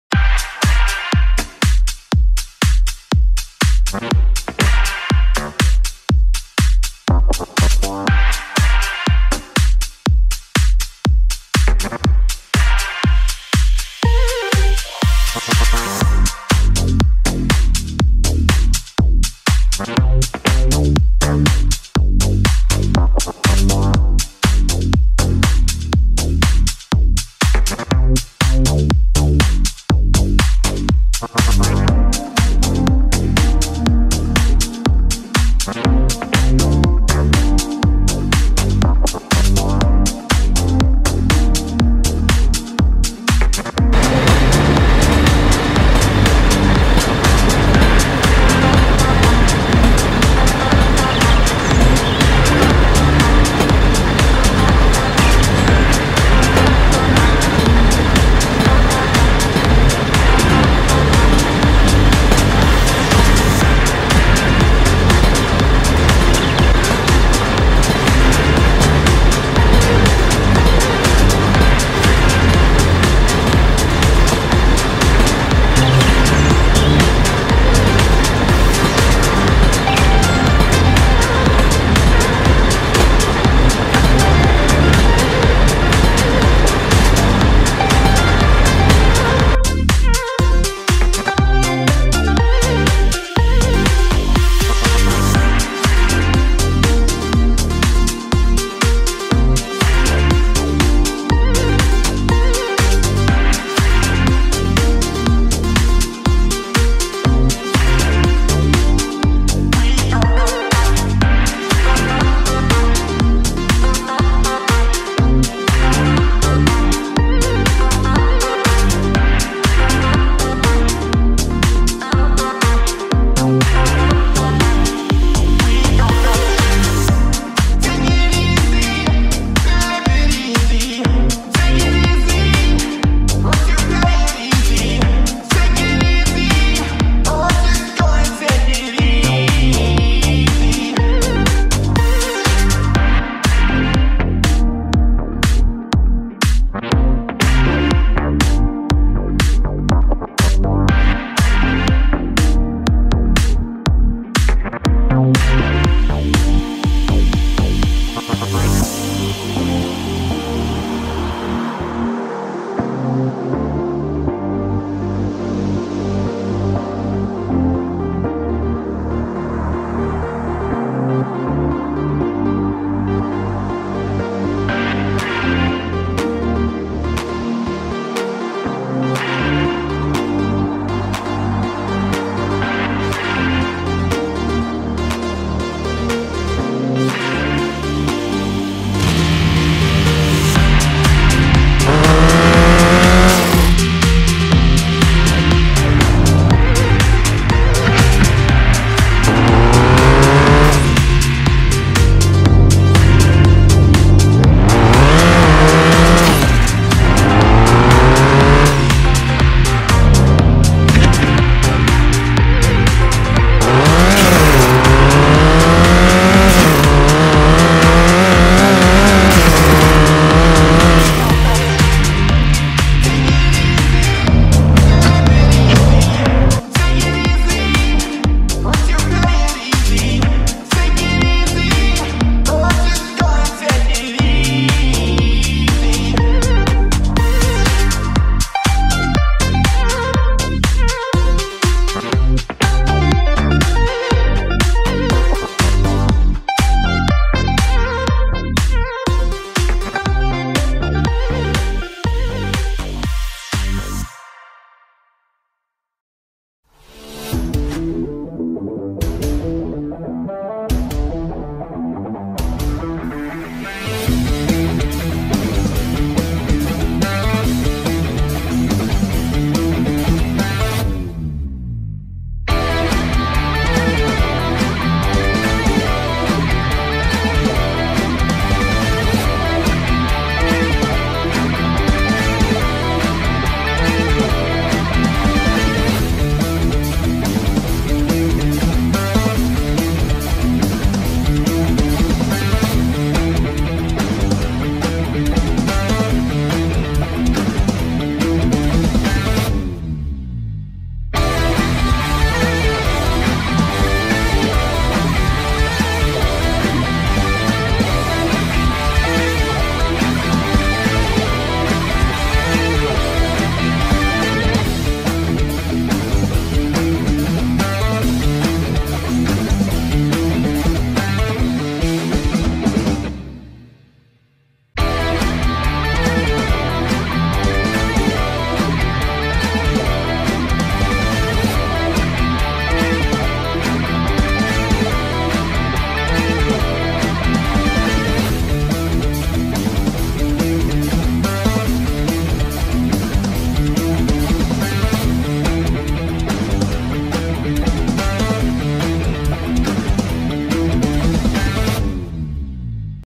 Driving Games NEWS! - Universal Truck Sim, Train Sim PRO USA, PetrolHead Traffic Quests, BeamNG
Let's see what's new in the Driving Games World.

Petrolhead Traffic Quests:
•Android: